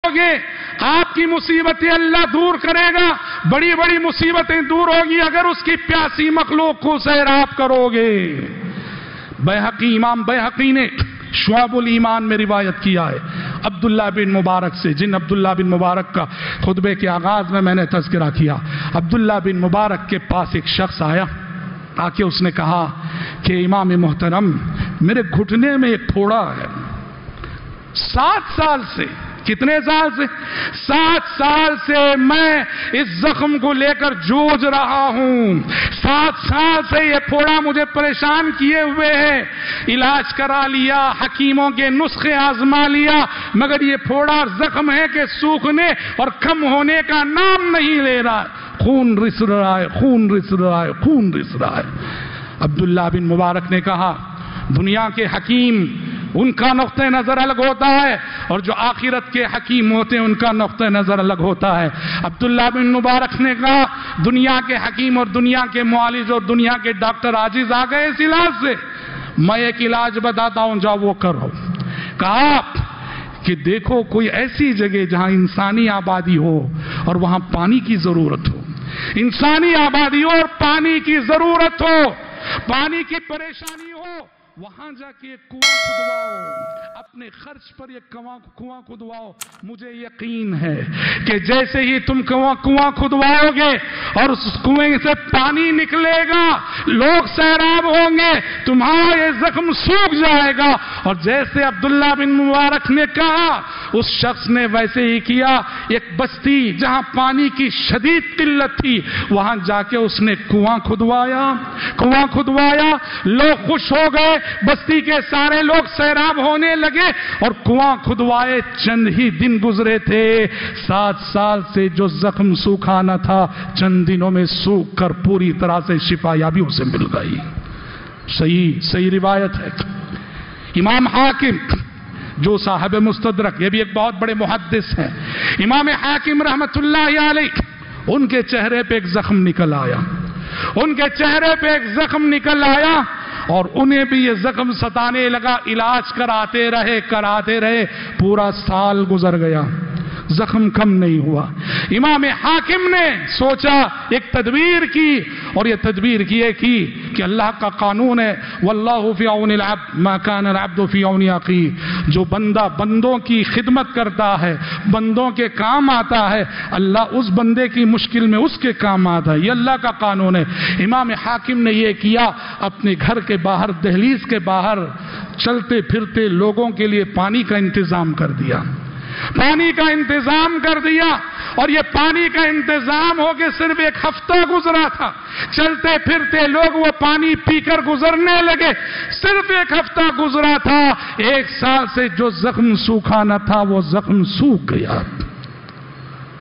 Pani pilane se musibat dur hoti hain | Shaykh Abdul Azeem Madani
This video clips is extracted from Jumu'ah Khutbah Pani Pilane Ki Neki by Shaykh Abdul Azeem Madani